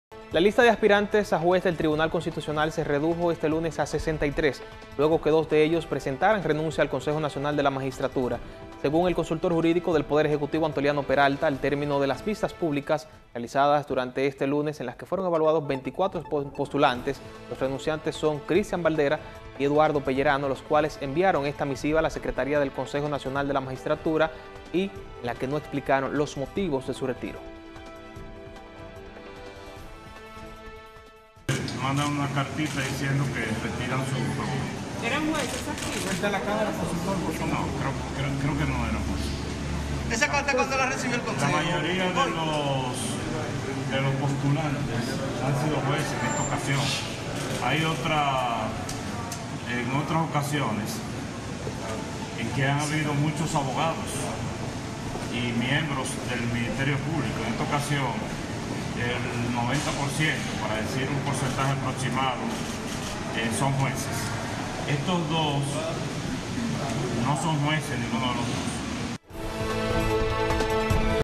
2 aspirantes al TC se retiran de evaluaciones del CNM.
La lista de aspirantes a juez del Tribunal Constitucional se redujo este lunes a 63 luego que dos de ellos presentaron renuncia al Consejo Nacional de la Magistratura (CNM). Según el consultor Jurídico del Poder Ejecutivo, Antoliano Peralta, al término de las vistas públicas realizadas este lunes en la que fueron evaluados 24 postulantes, los renunciantes son Cristian Baldera y Eduardo Pellerano, los cuales enviaron una carta a la secretaria del CMN, magistrada Nancy Salcedo, en la que no explican los motivos de su retiro